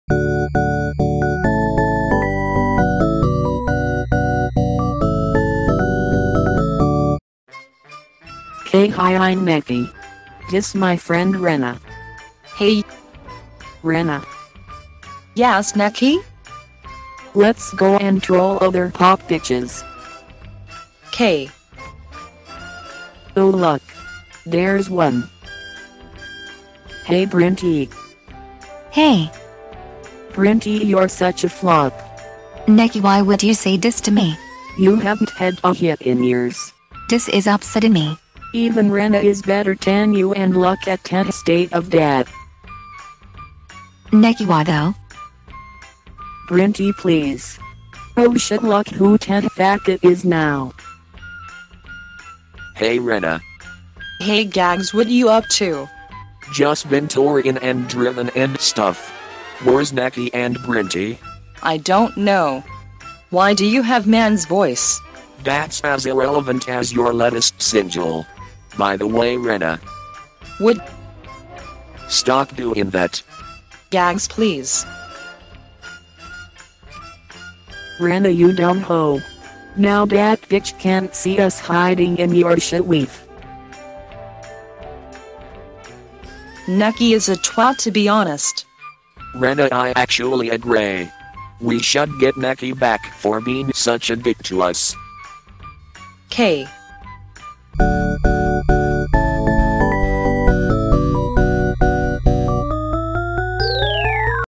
The Nekci Menij Show - Episode 1 - Trolling Brinty
Nekci Menij introduces the borbz to her friends.